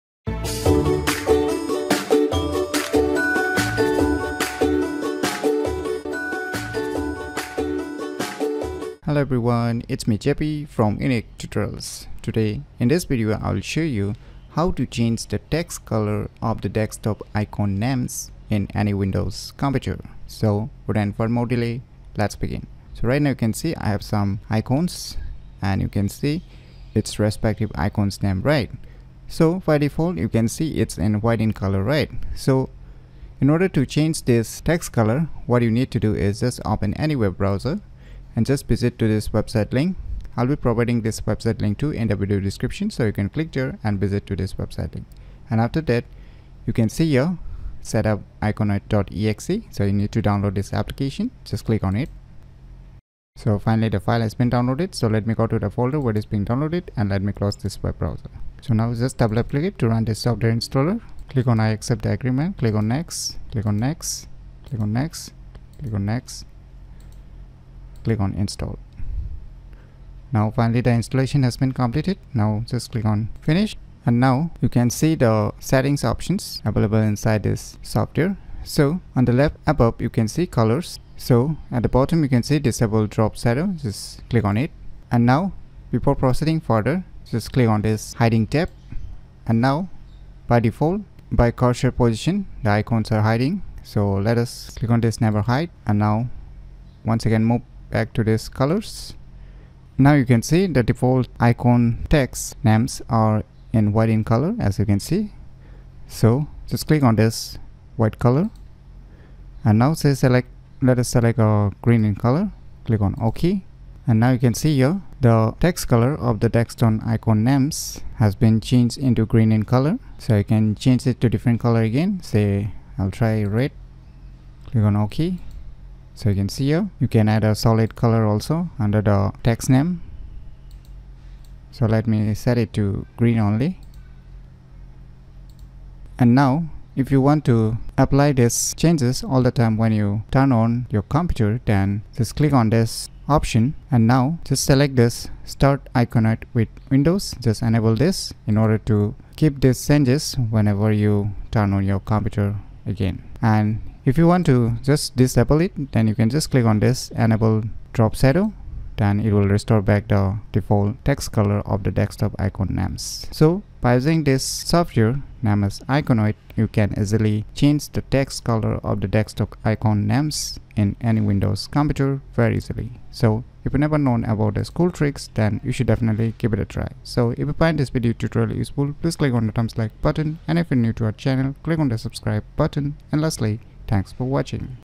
How to change the Text Colour of the Desktop Icon Names in any Windows computer ?
This video tutorial is all about How to change the Text Colour of the Desktop Icon Names in any Windows computer.